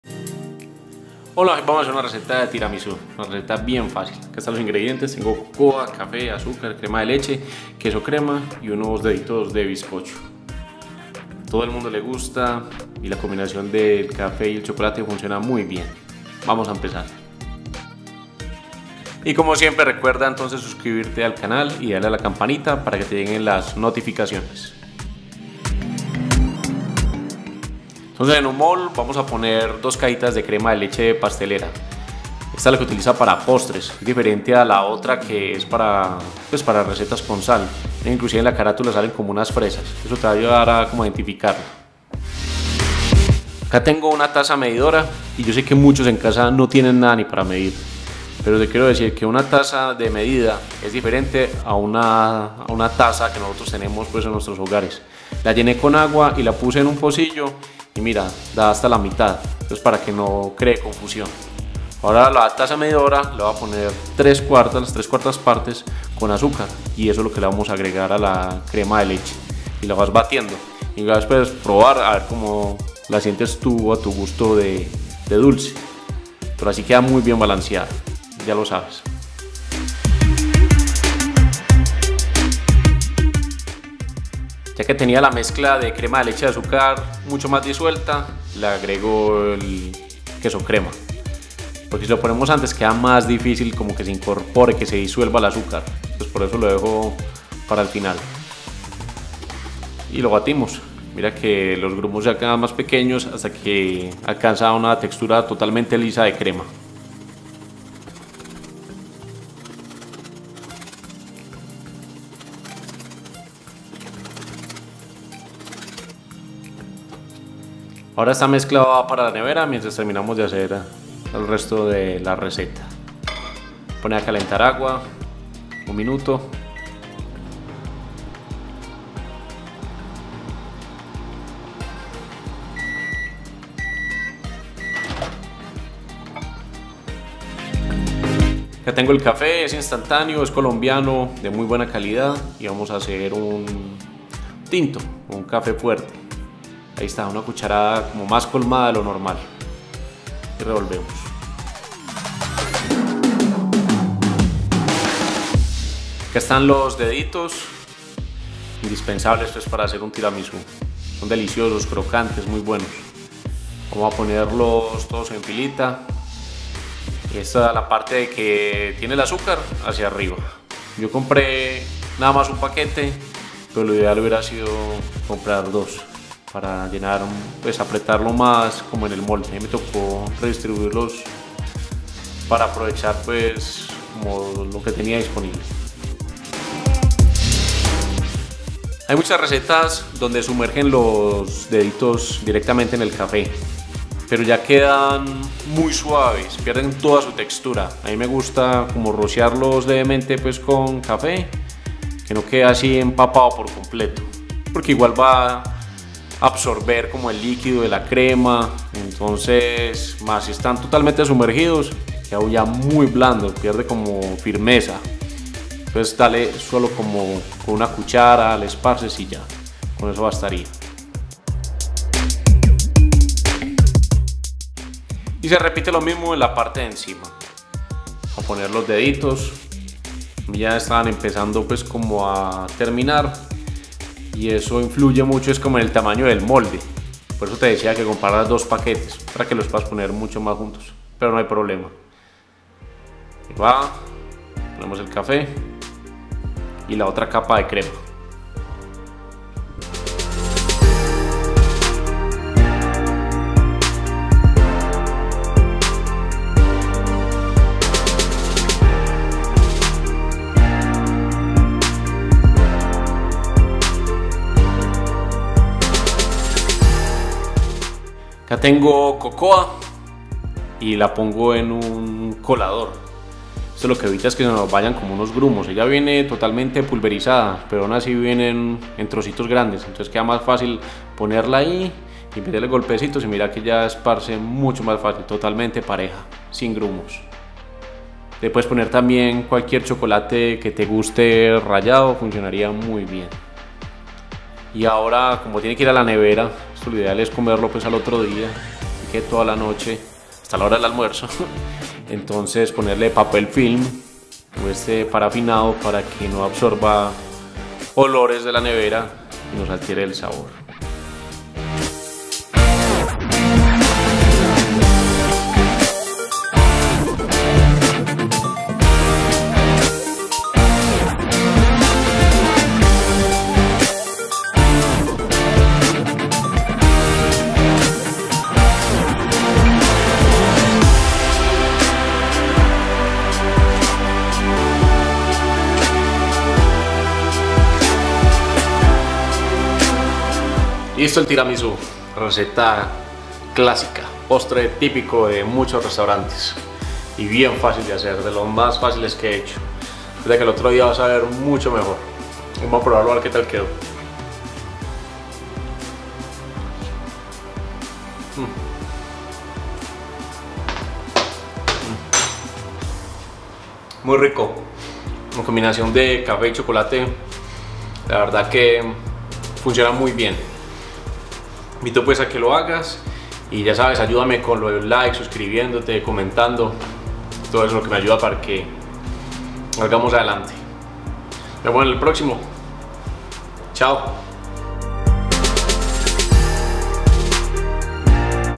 TIRAMISU CASERO | Receta de TIRAMISU fácil (#81)
Receta de Tiramisú Casero
Un postre clásico italiano que nos gusta a todos

Ingredientes:
1 tarro de deditos
1 tarro de queso crema
2 cajitas de crema de leche
3/4 de azúcar
1 taza de tinto
2 cucharadas de cocoa